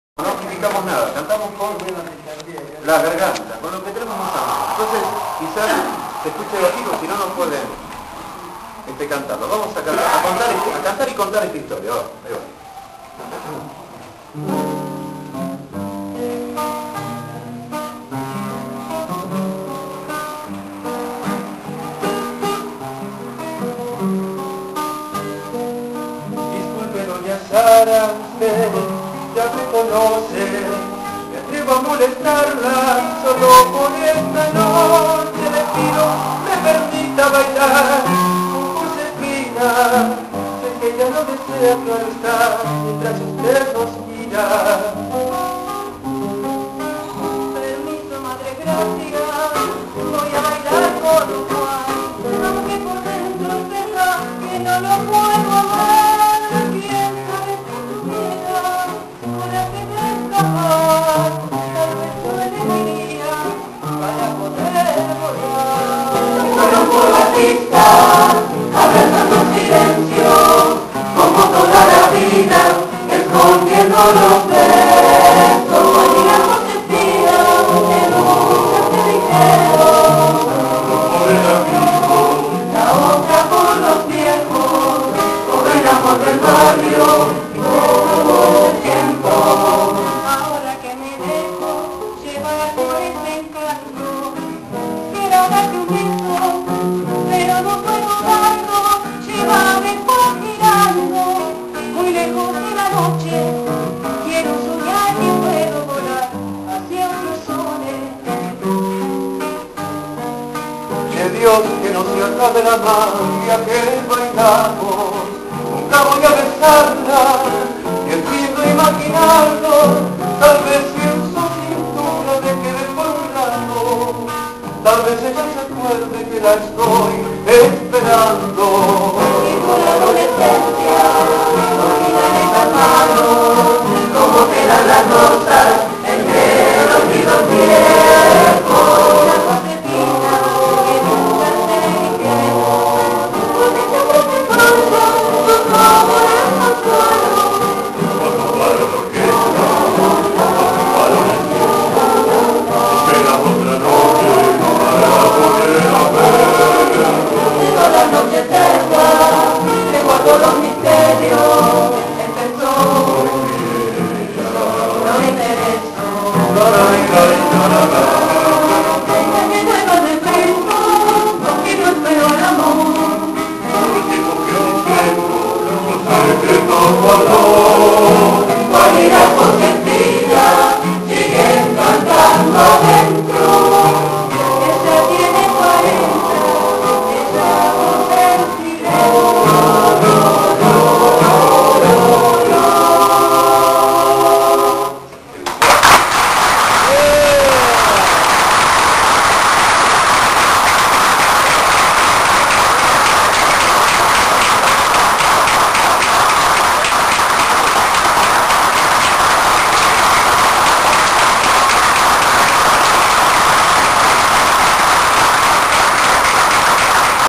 Coro municipal de Almirante Brown
Coro municipal de Almirante Brown en su excelente actuación en la Feria del Libro de Claypole, a cargo de Lito Otero.